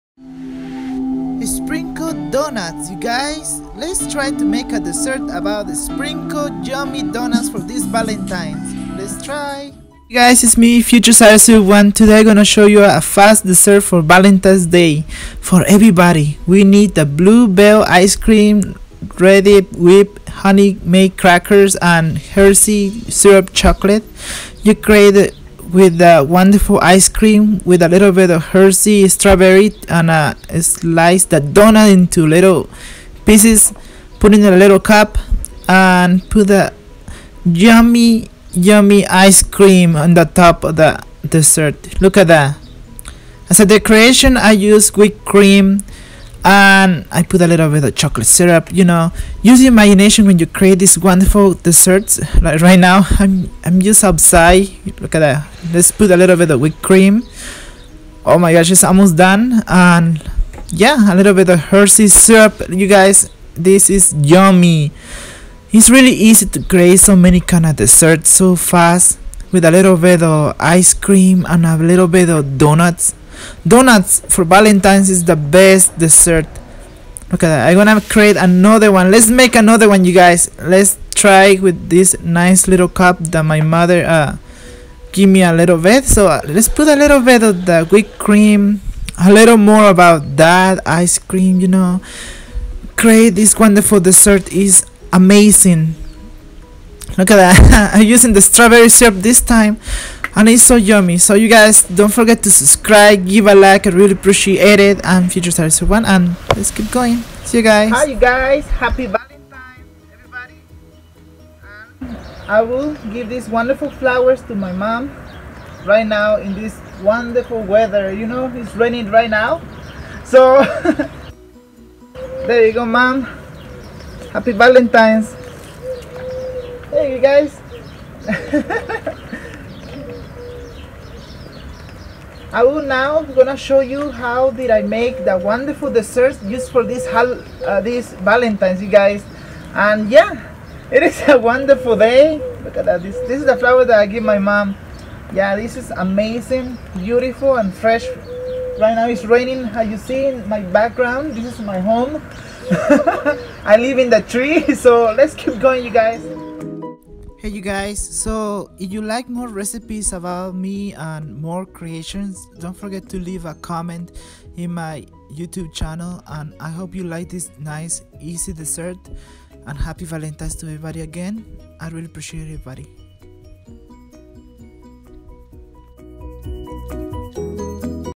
Valentine’s Day Sprinkled Donut 🍩 Fast Dessert
Best and fast dessert in Valentine’s Day and yummy 😋 I hope you like my favorite dessert and enjoy 😉